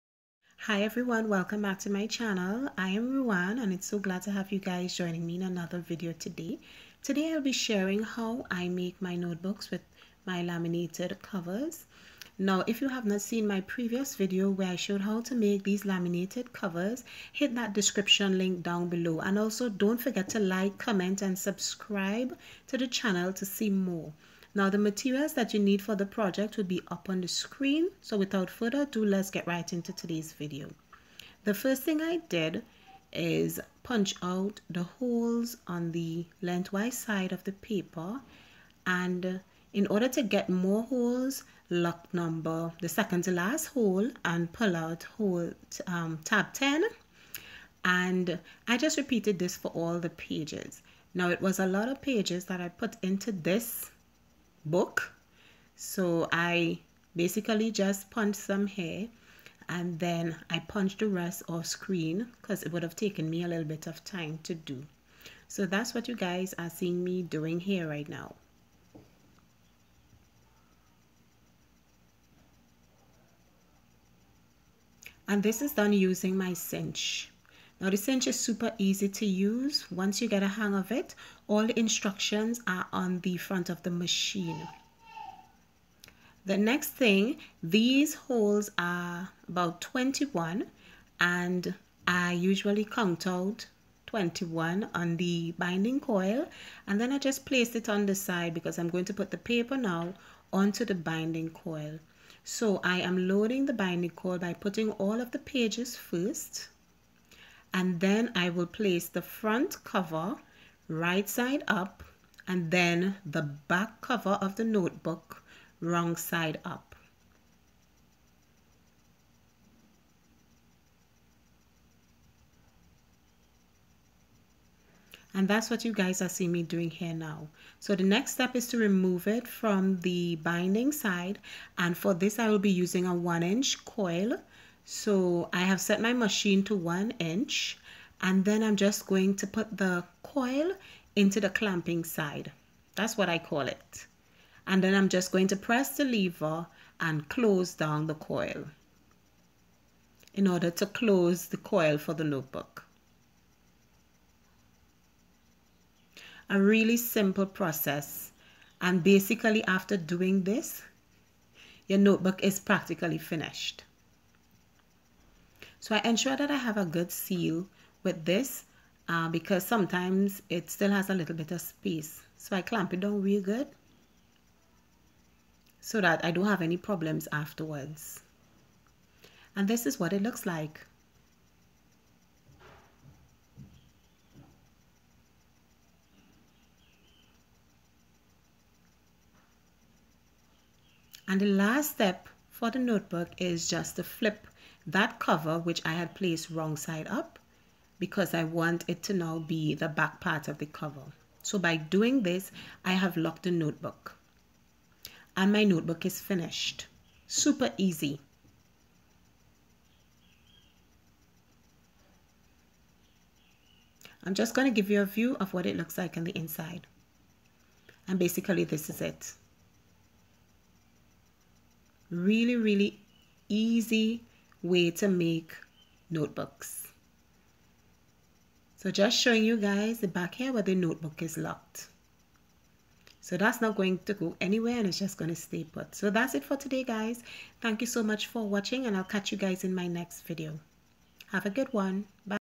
How to make a notebook with a laminated cover | We R Memory Keepers Cinch Binding Tool
Hello, in this video I share how to make notebooks with laminated covers. Hope you enjoy my video.

I would love to connect!

All items used in this video were purchased by me, unless otherwise stated.